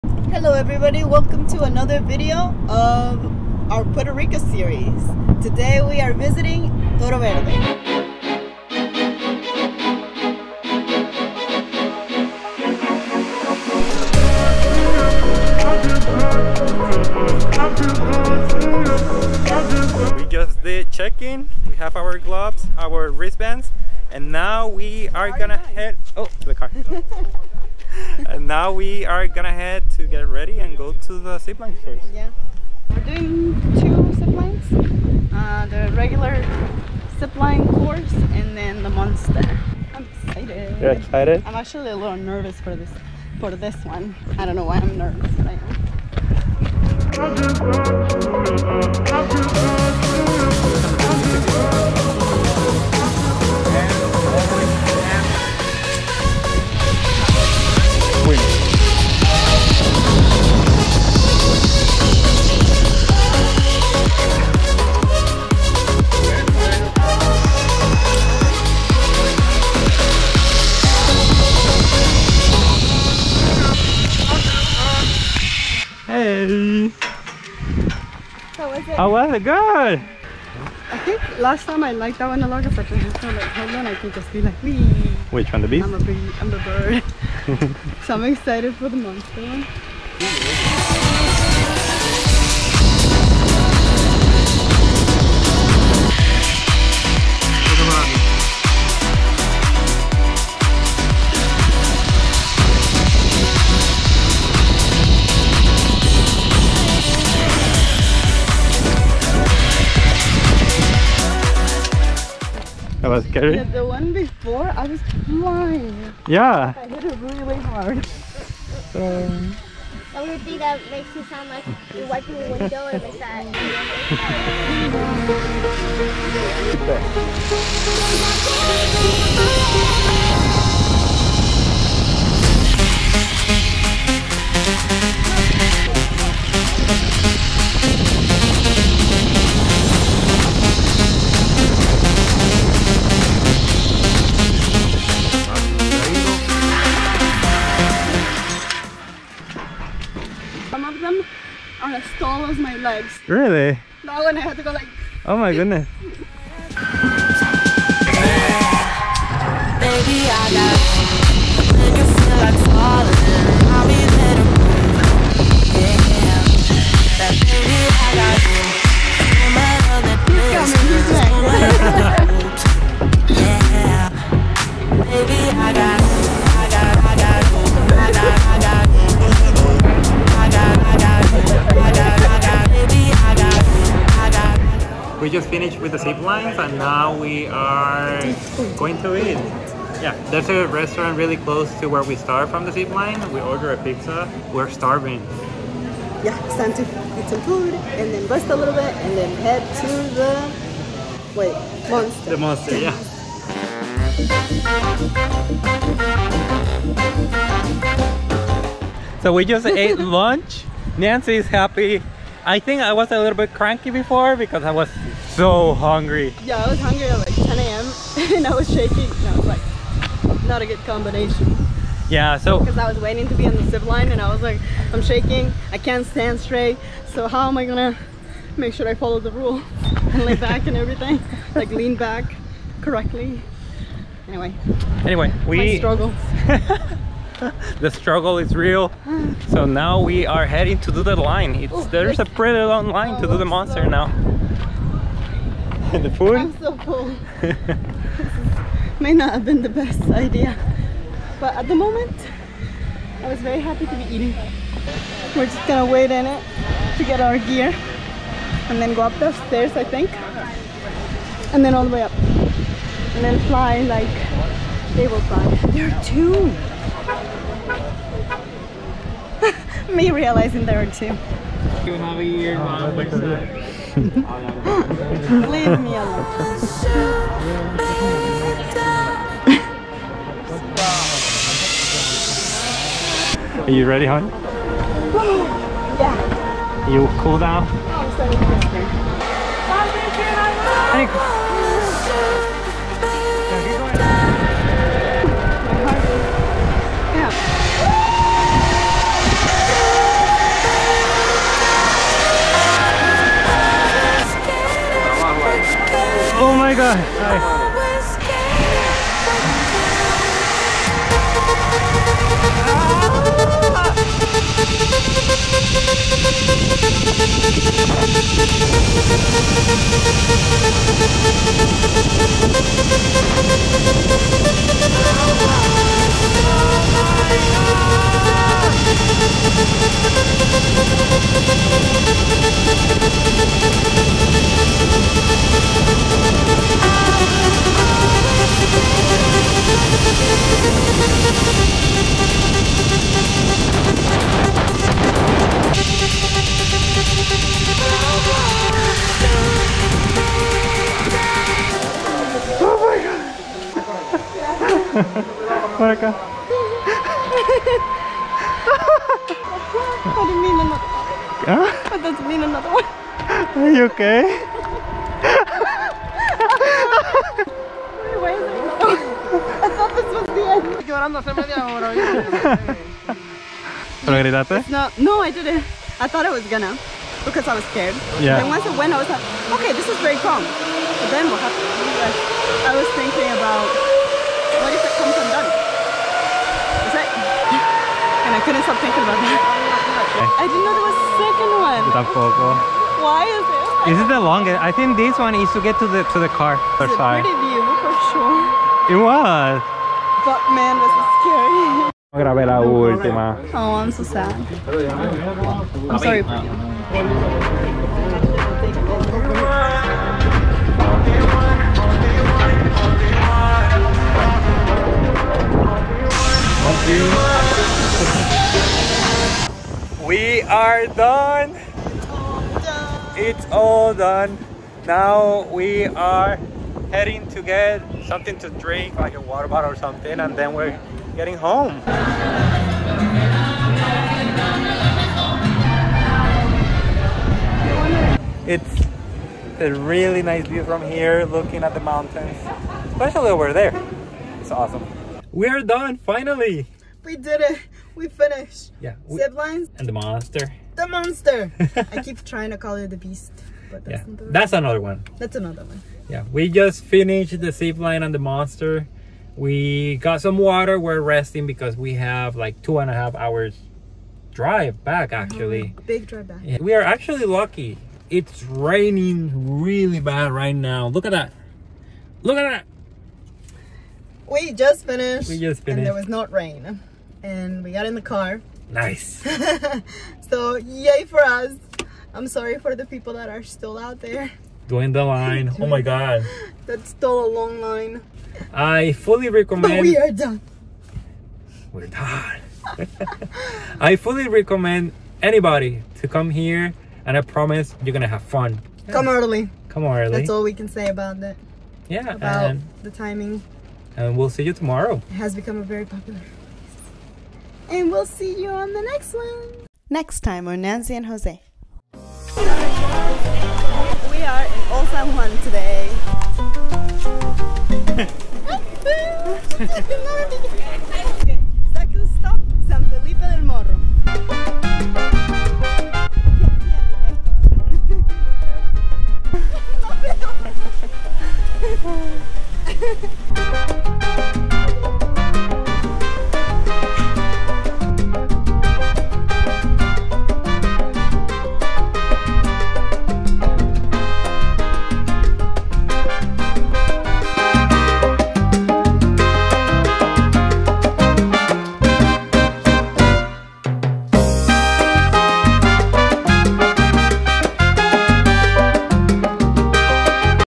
We are in PUERTO RICO! - Part 3 | Toro Verde Adventure Park | Puerto Rico Travel Guide and Vlog
Welcome to Toro Verde, Puerto Rico's ultimate adventure park! Located in the lush mountains of Orocovis, Toro Verde offers an unforgettable experience for thrill-seekers and nature lovers alike. In this video, we'll take you on a virtual tour of Toro Verde, showcasing some of the park's most exhilarating attractions. Whether you're a seasoned adrenaline junkie or just looking for a unique way to experience Puerto Rico's natural beauty, Toro Verde is the perfect destination. So come along with us and discover the thrills and wonders of this incredible park!